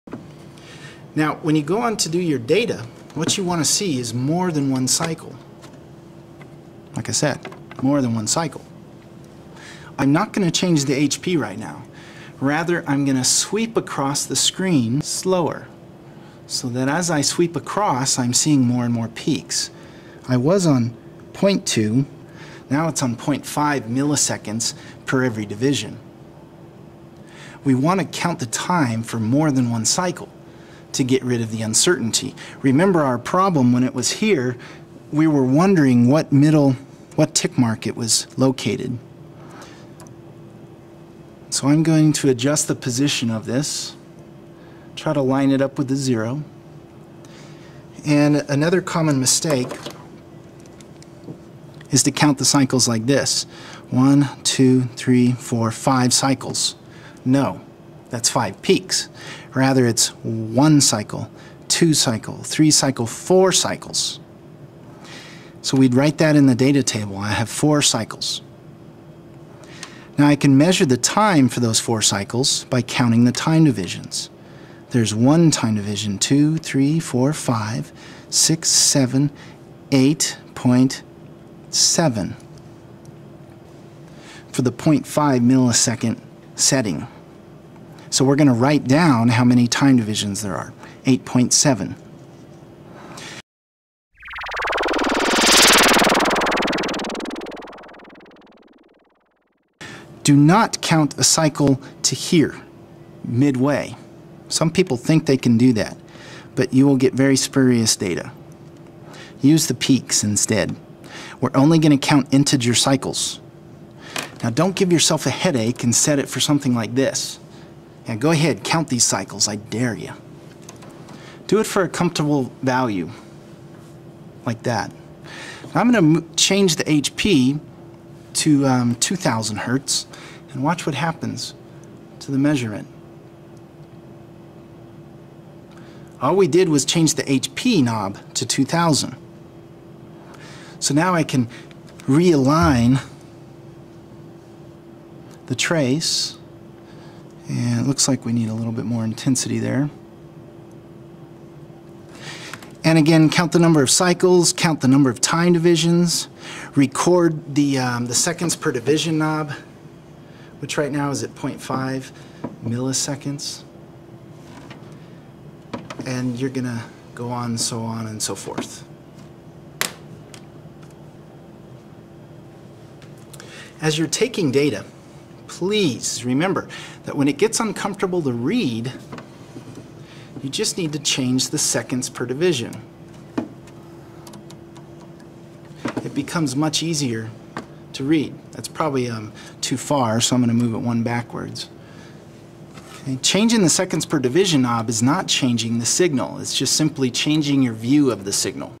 Oscilloscope, Week 1: Part 4, Data Taking
How to take data for the Oscilloscope Lab Experiment.